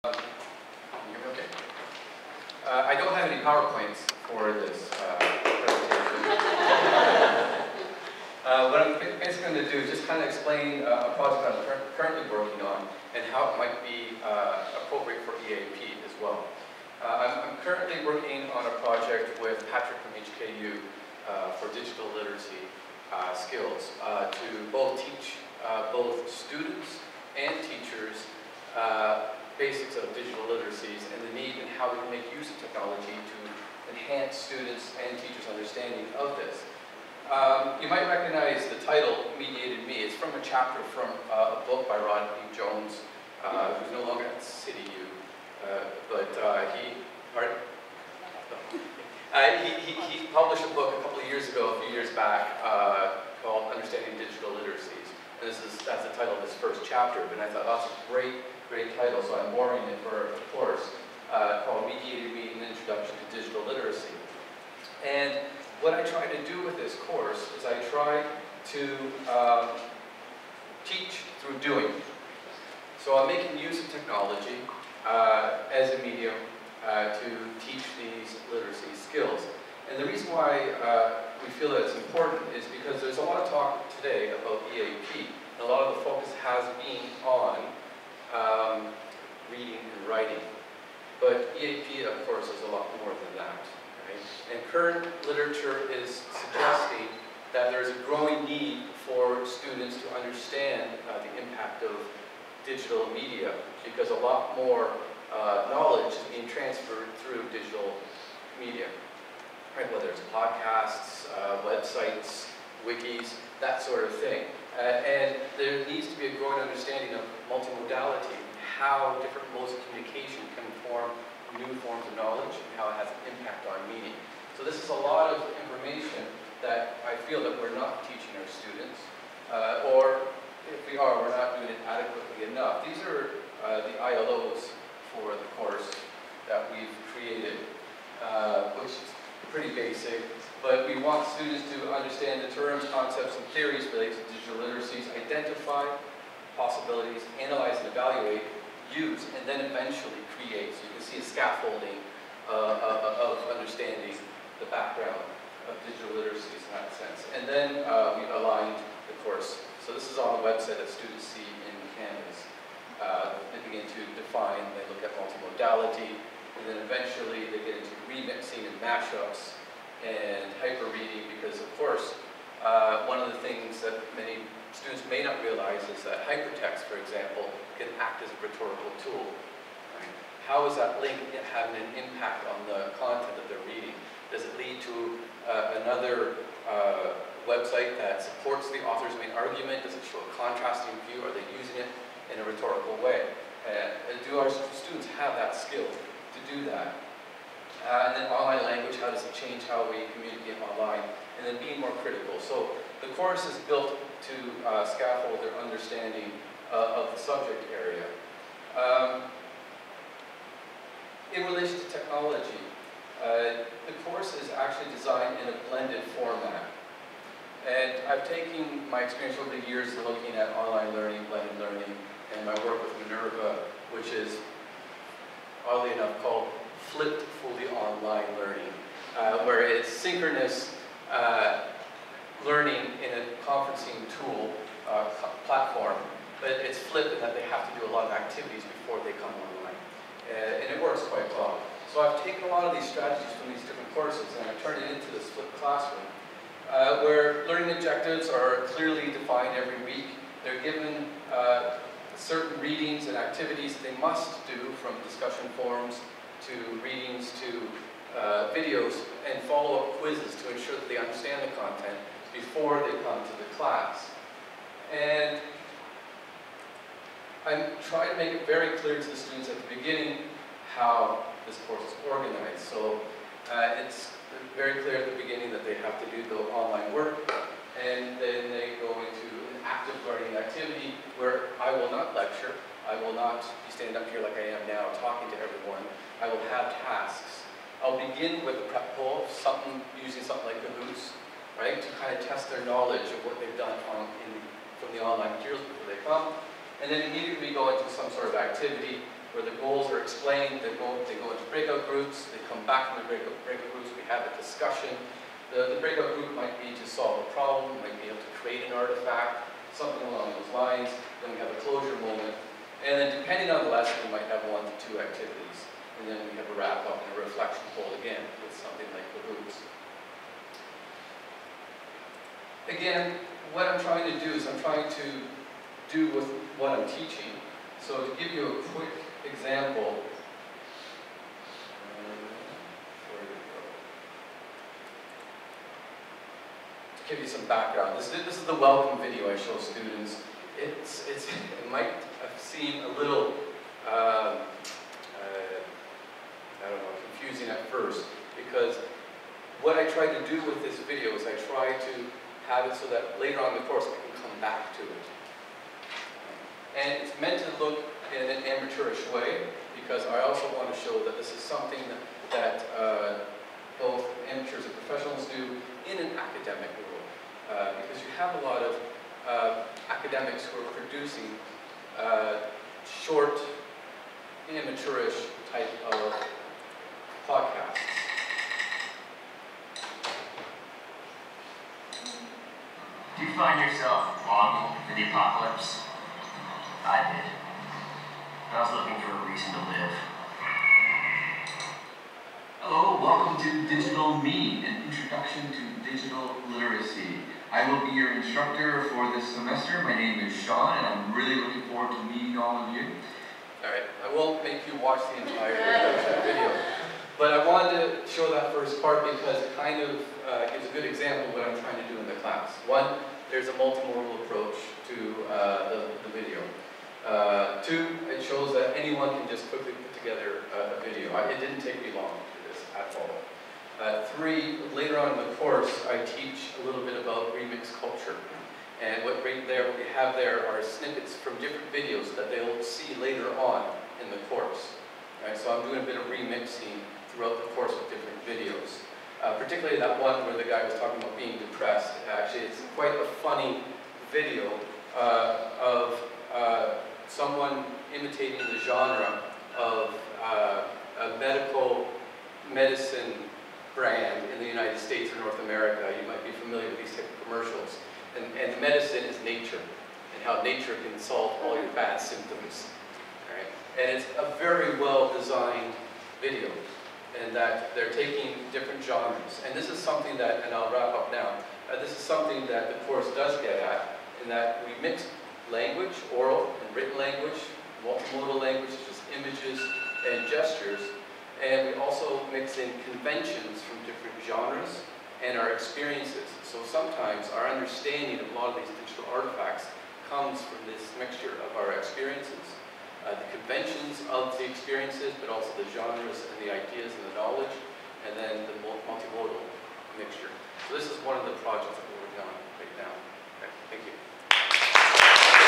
Sean McMinn (HKUST )
Recorded at the colloquium on “Inquiry into expertise in EAP teaching: EAP teachers' professional growth”

26 January 2018 (Saturday)
10 am - 1 pm
The University of Hong Kong, Centennial Campus,  LG/F Central Podium, CPD-LG.61 and CPD-LG.62

to see more videos, and more resources.

Click "Subtitles/Closed captions" for English subtitles.